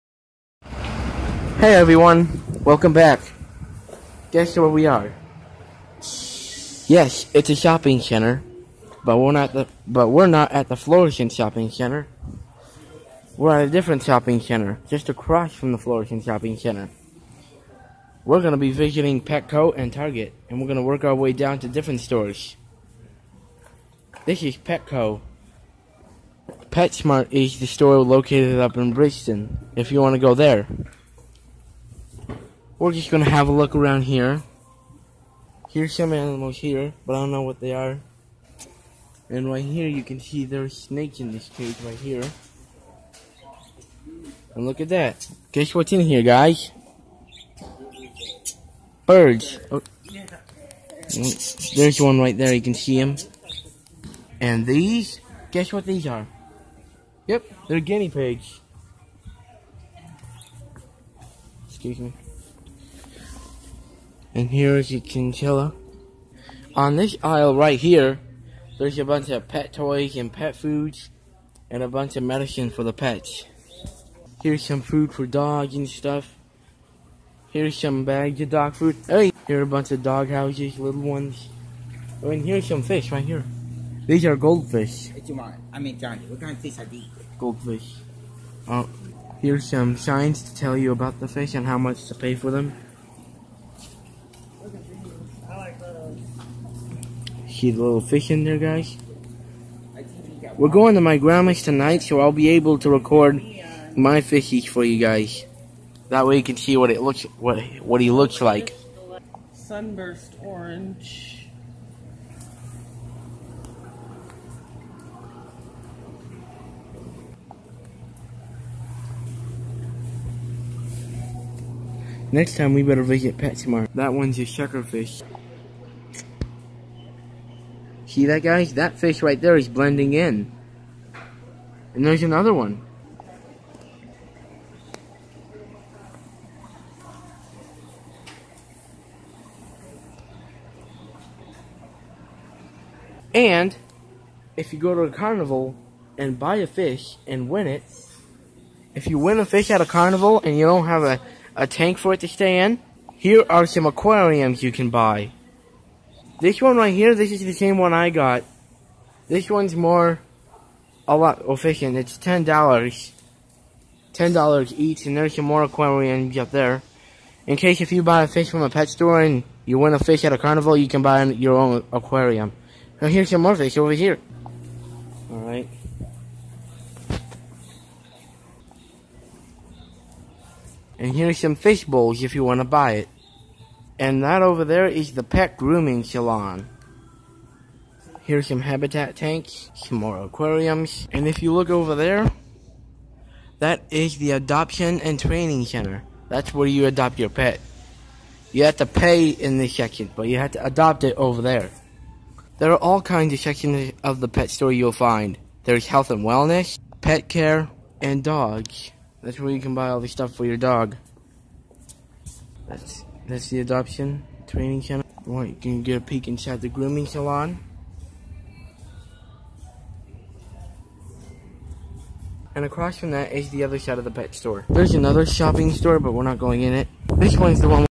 Shopping center part 1 Petco
Guess we'll ride today guys, it's not Florissant Square shopping center, no, we are at a different shopping center, just next to Aldi and close to Florissant Square shopping center, the stories we will be visiting today or Petco Target and Five Below, the first store going to go visit Petco